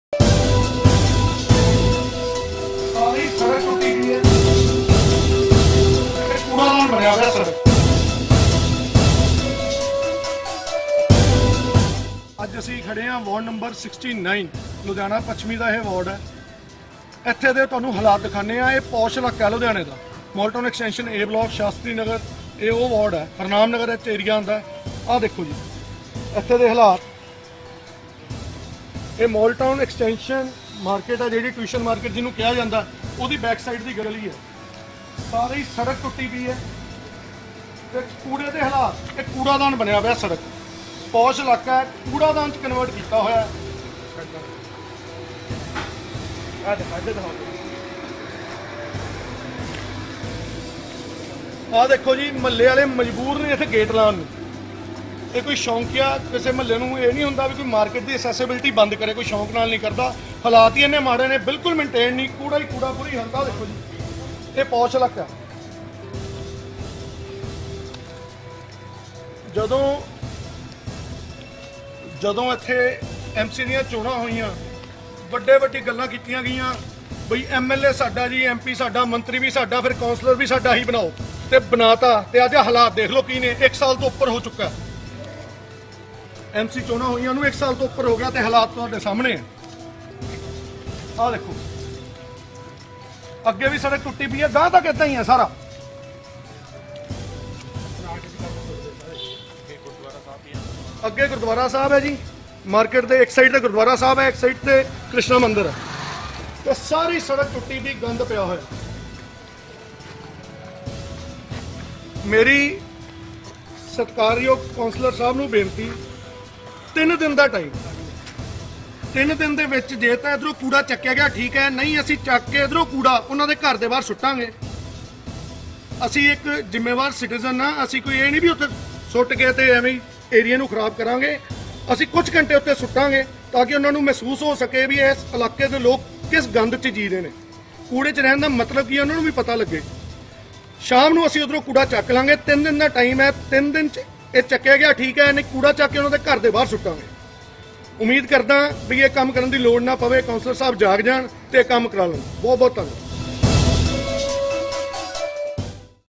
Gurkaran Singh Tinna
ਵਾਰਡ ਨ. ੬੯ ਦੇ ਕੌਂਸਲਰ ਨੂੰ ਲੁਧਿਆਣਾ ਵਾਸੀ ਗੁਰਕਰਨ ਸਿੰਘ ਟੀਨਾ ਦੀ ਧਮਕੀ
Ludhiana Resident Gurkaran Tinna's Threat to Ward No. 69